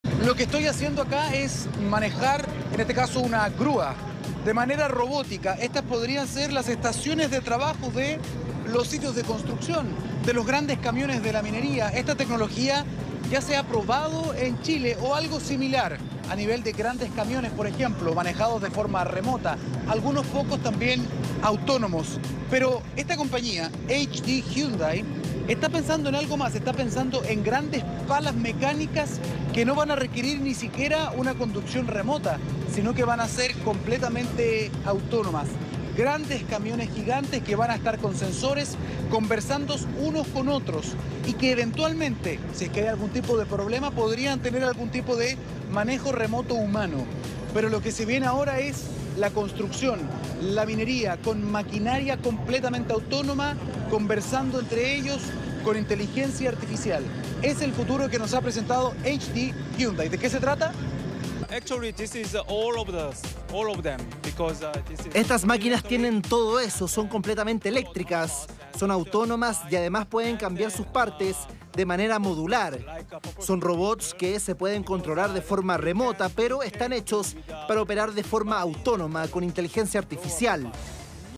Así serán los sitios de construcción en el futuro: Maquinaria ya puede ser manejadas a distancia
Conocimos los últimos avances en la tecnología de maquinaria pesada y experimentamos cómo serían los sitios de trabajo en un futuro cada vez menos distante.